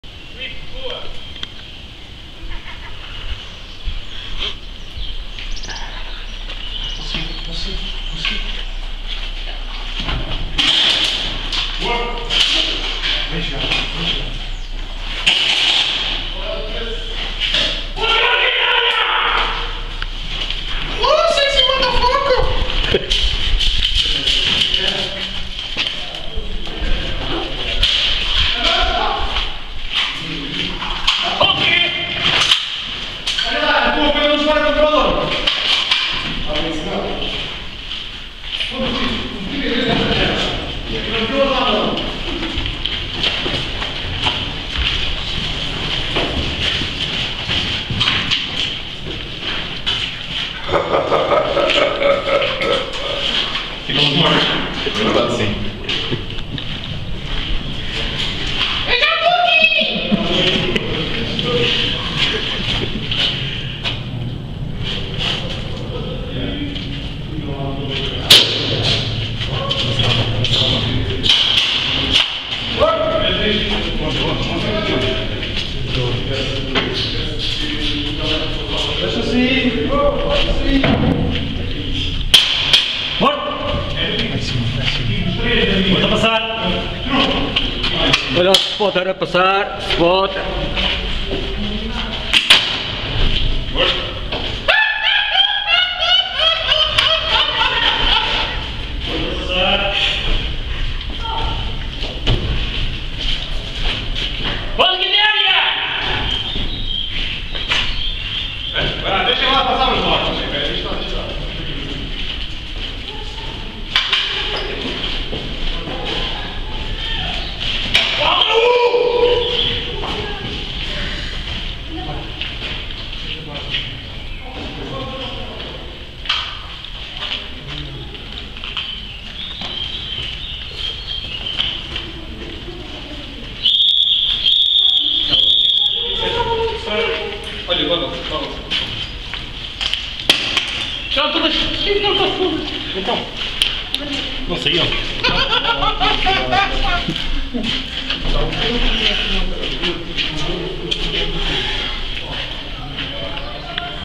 Pentaforce Templarius e Convidados - 07-06-2014 - 03-03
Uma das entradas que dá acesso ao túnel da prisão.